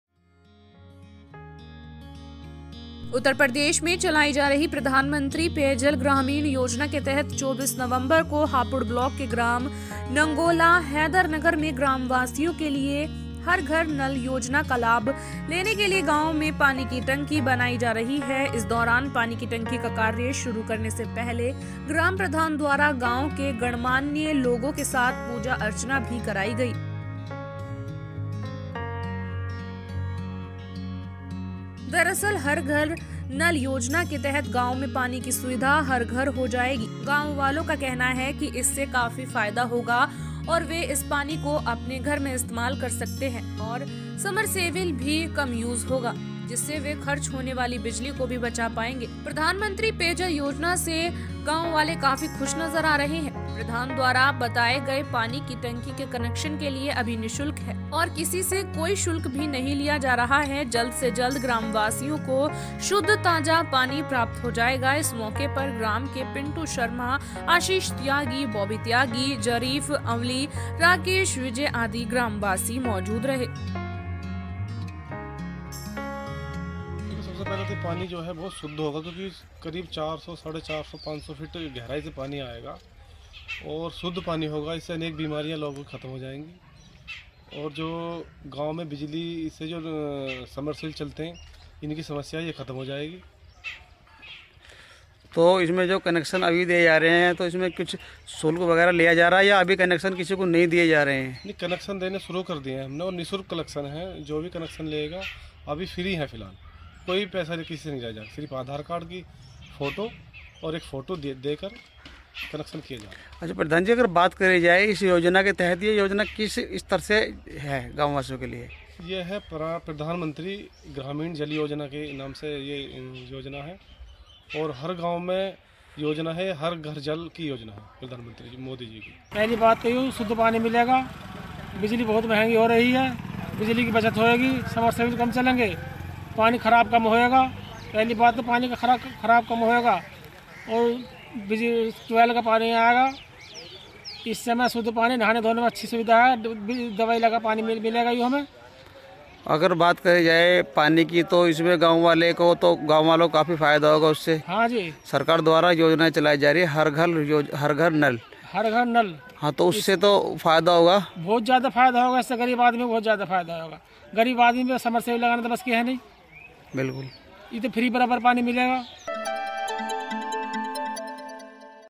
‘हर घर नल-हर घर जल’ योजना से ग्रामवासियों को जल्द मिलेगी राहत
हापुड। उत्तर प्रदेश में चलाई जा रही प्रधानमंत्री पेयजल ग्रामीण योजना के तहत 24 नबंवर को हापुड़ ब्लाक के ग्राम नंगोला हैदरनगर में ग्राम वासियों के लिए हर घर नल योजना का लाभ लेने के लिए गांव में पानी की टंकी बनाई जा रही है। इस दौरान पानी की टंकी का कार्य शुरू करने से पहले ग्राम प्रधान द्वारा गांव के गणमान्य लोग के साथ पूजा अर्चना भी कराई गई।